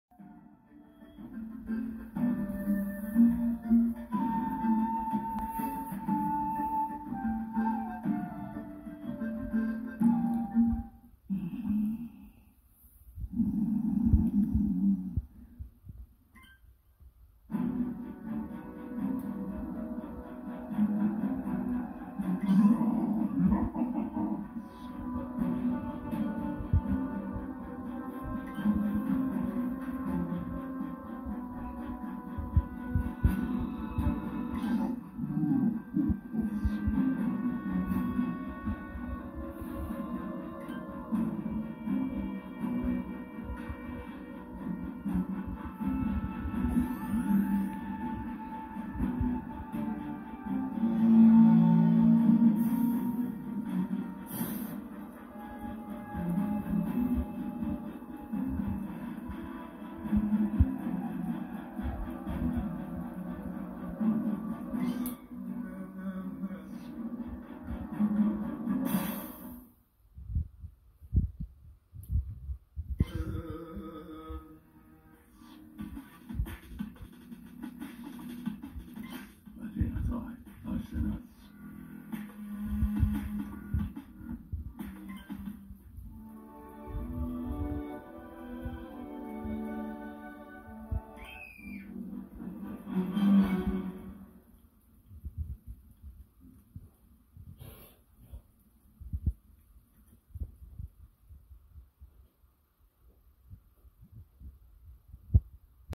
The SpiderMan and Batman Show: Sleeping in Car
Jam and Joker Fight Each other while SpiderMan is Sleeping.